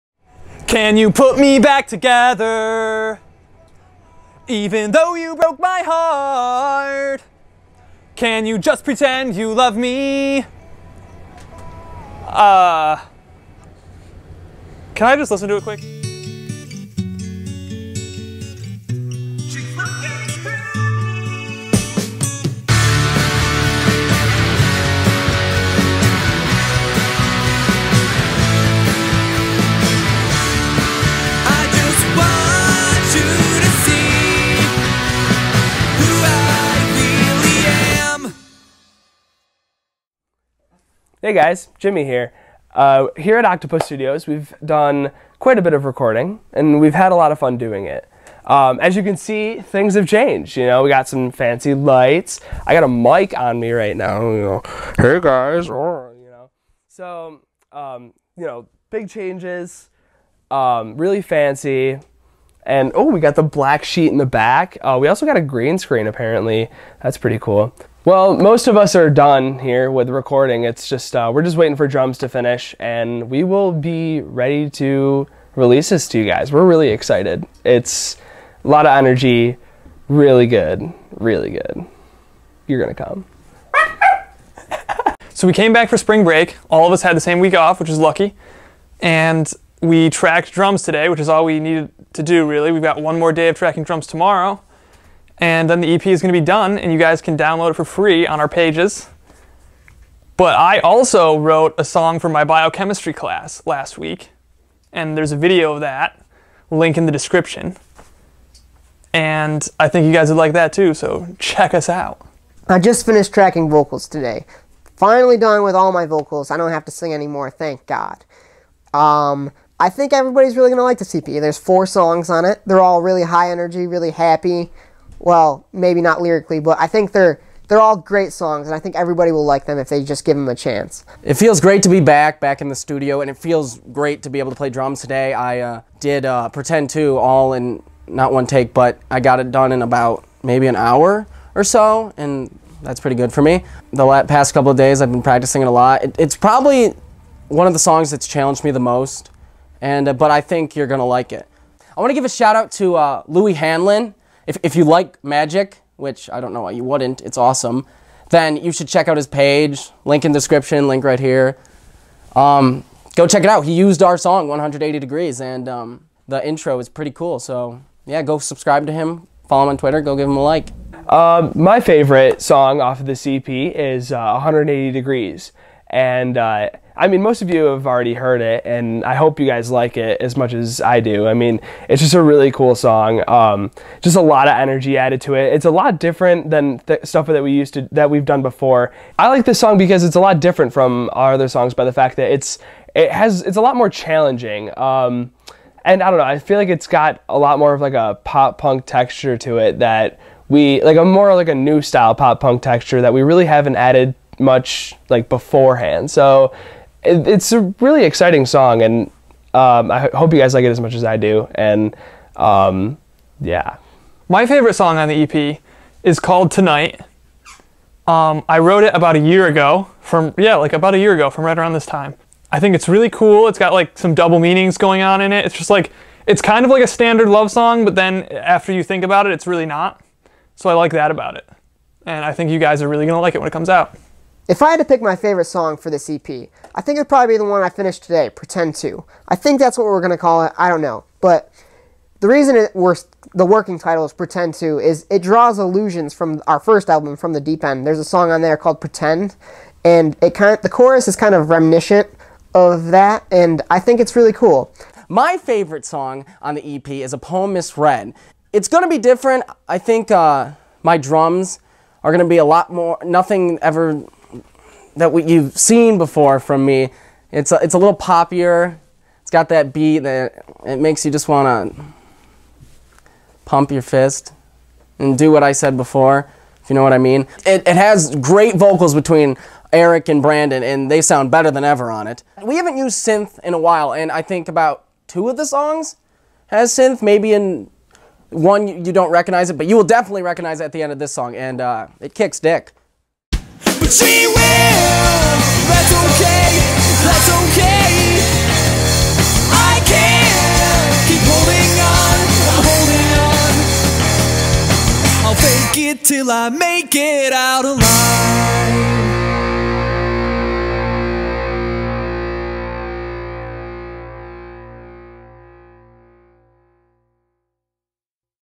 The Studio - Finishing Up
Episode 7 - Finishing Up. We discuss finishing up drums and vocals, putting the finishing touches on the new EP, a shoutout to a good friend of ours, and our favorite songs on the new EP!